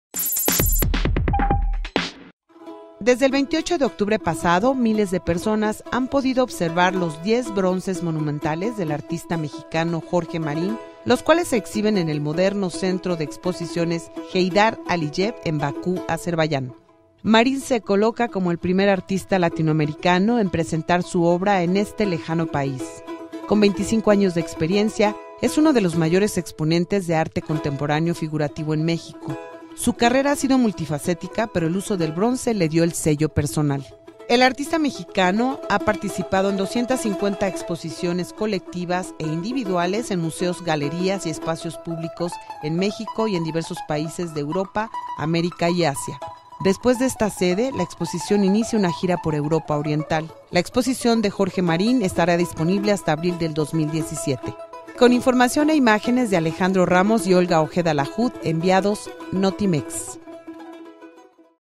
Miles de personas visitan exposición del escultor mexicano Jorge Marín en Azerbaiyán
El centro Aliyev, que albergará por seis meses la exposición de Jorge Marín, hasta abril de 2017, fue diseñado por la arquitecta Zaha Hadid, reconocida con el prestigioso premio de arquitectura